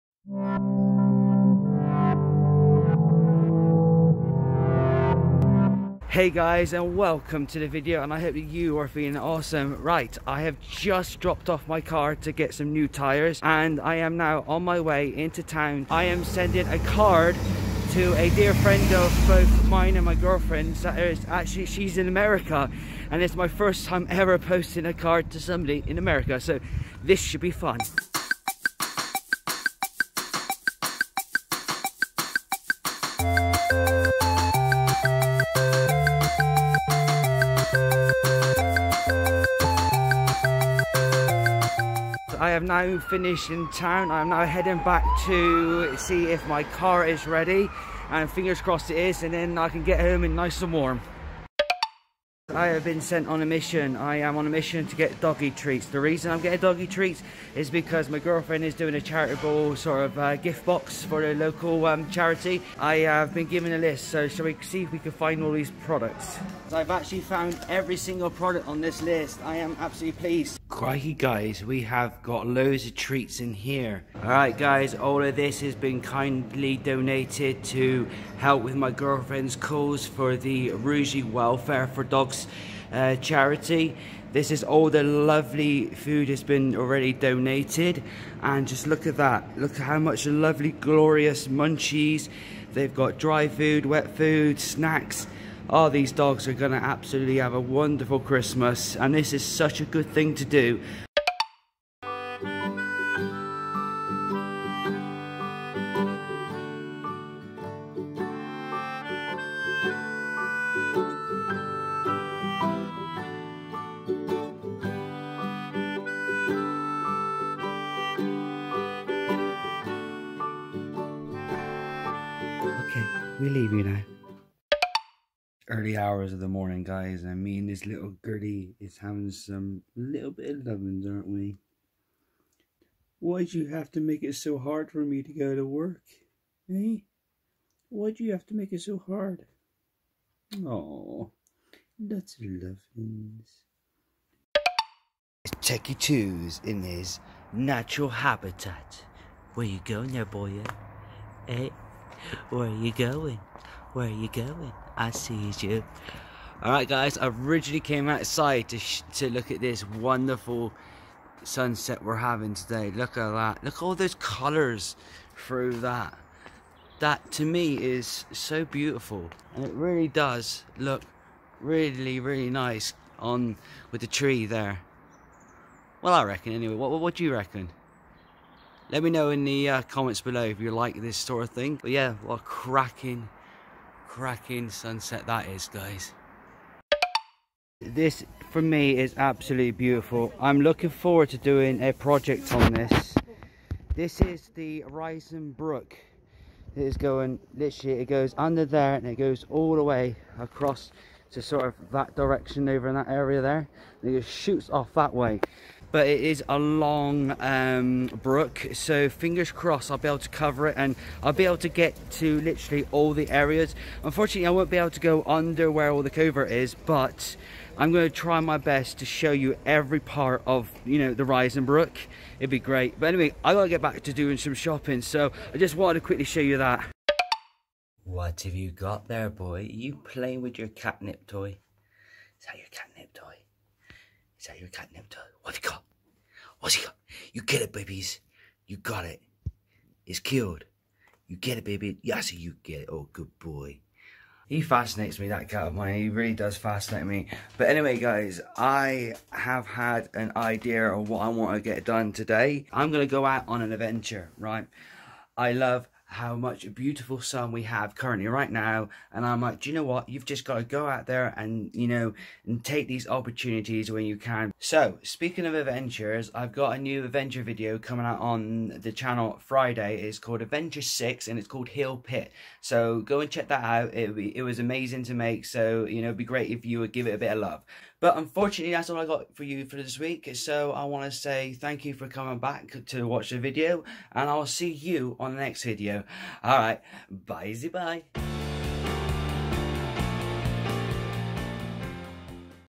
Jon's Weekly Vlog 9: Doggy Donations - Rugeley
In this weeks video we helped with donating some food to a local Dog charity and I told you future plans of more content.

Thank you for visiting and see you soon